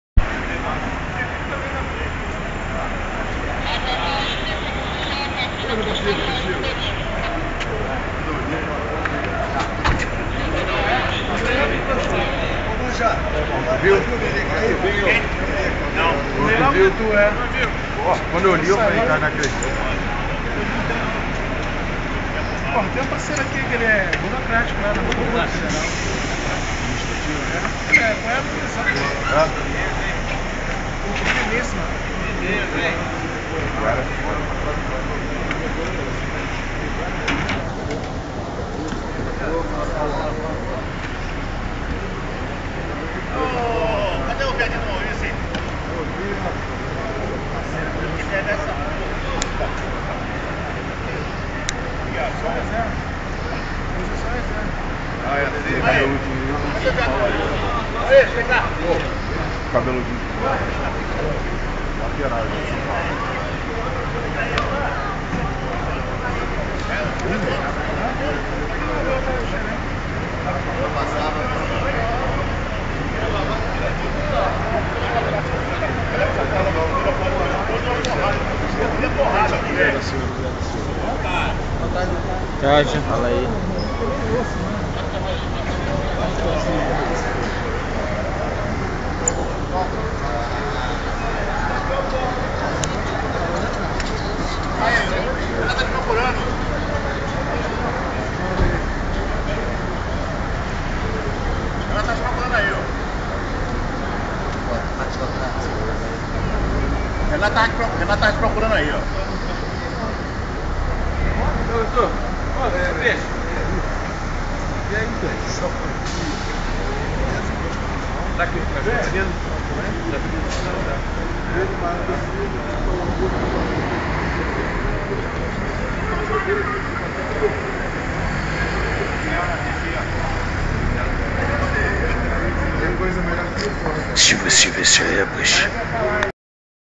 FLUMINENSE CHEGANDO AO MARACANÃ. 18/05/2008
FLU DE RENATO GAUCHO CHEGANDO AO MARACANÃ.